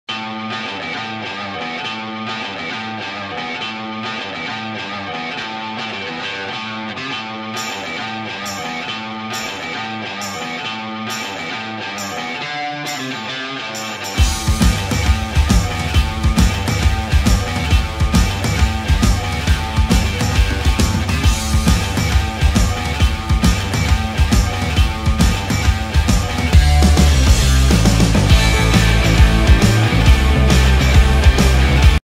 epic nextbot running
couldn't find better music. Only reason I uploaded this was because I jumped past sanic. I also intended this to be a youtube short but I don't know how. And I couldn't find better music